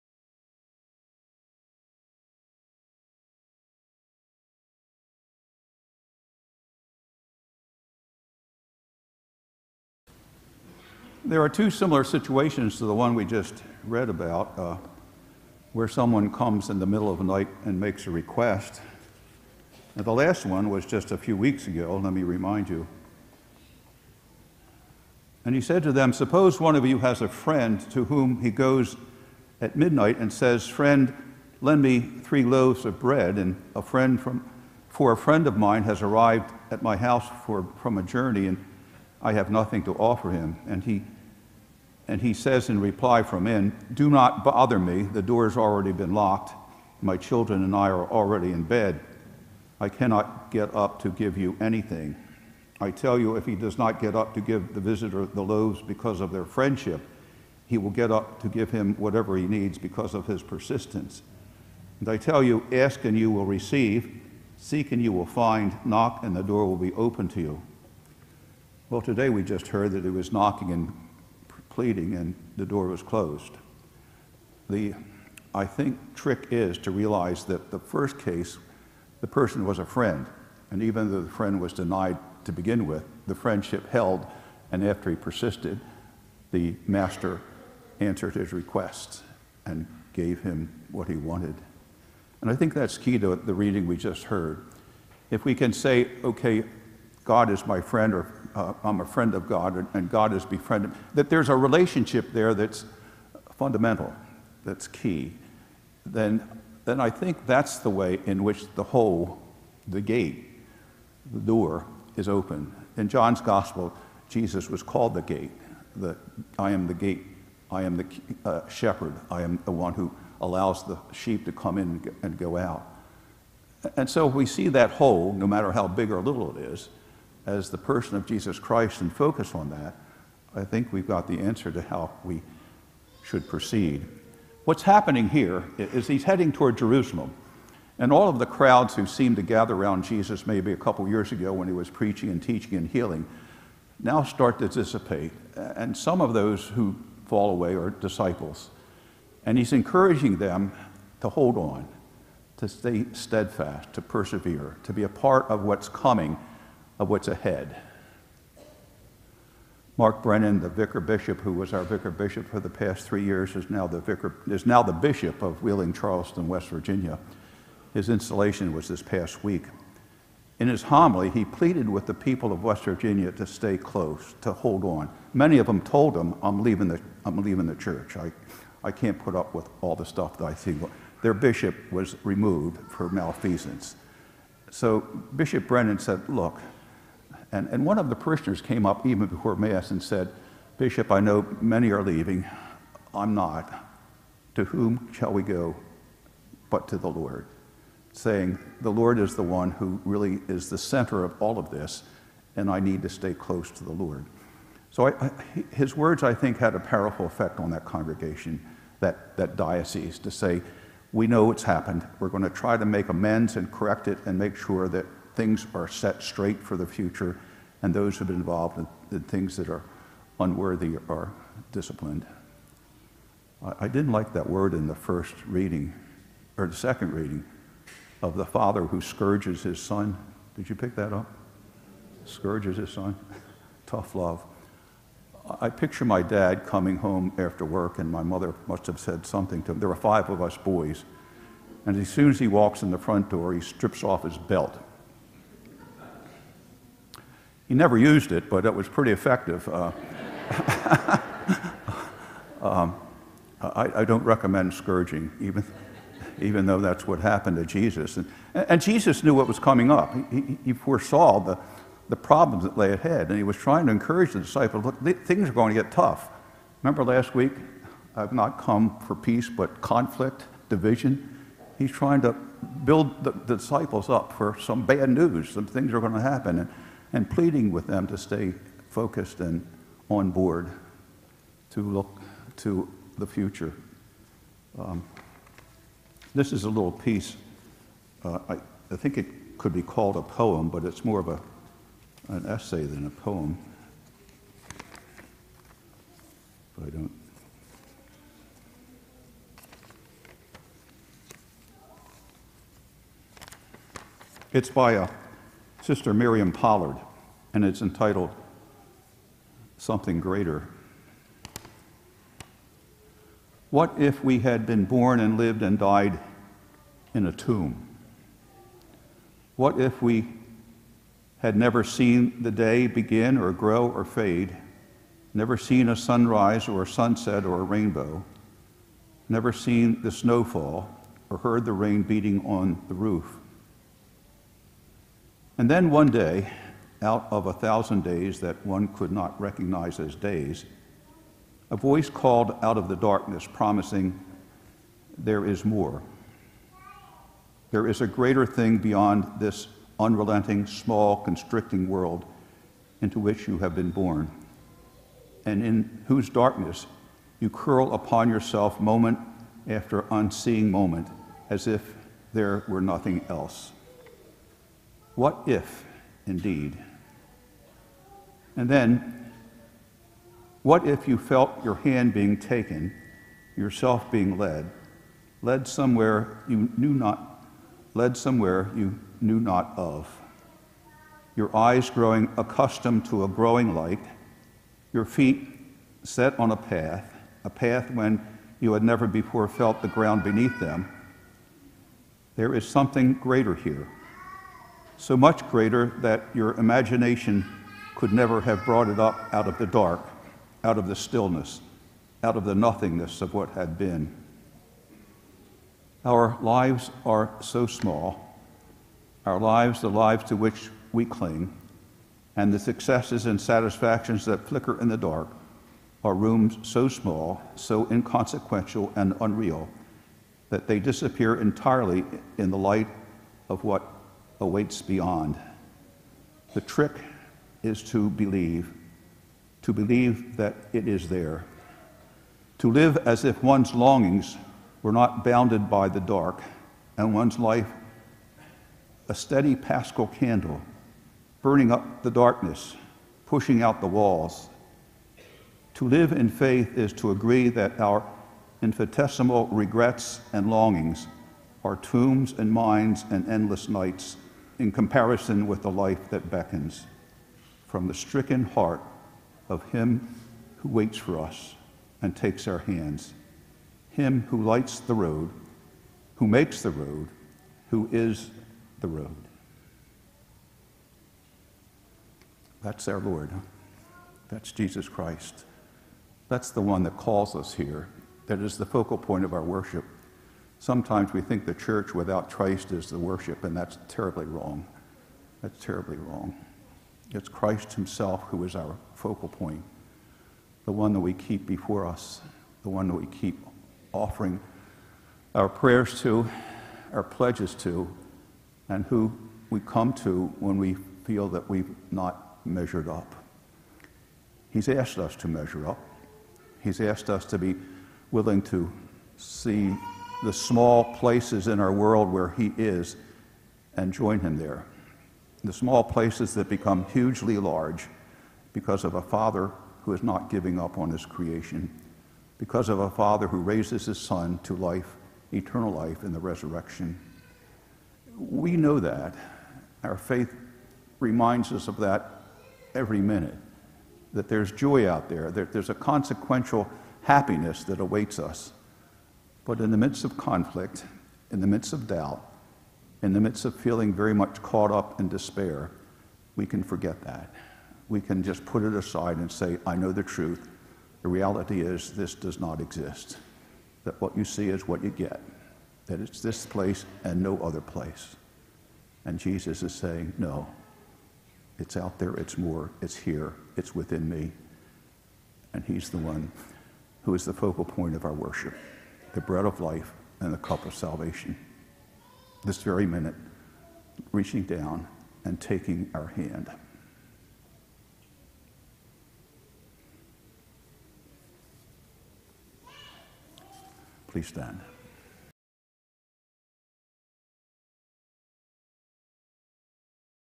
2019 08 25 1030 FrTillman Homily
Msgr. Tillman 1030 am Mass
Twenty-First Sunday in Ordinary Time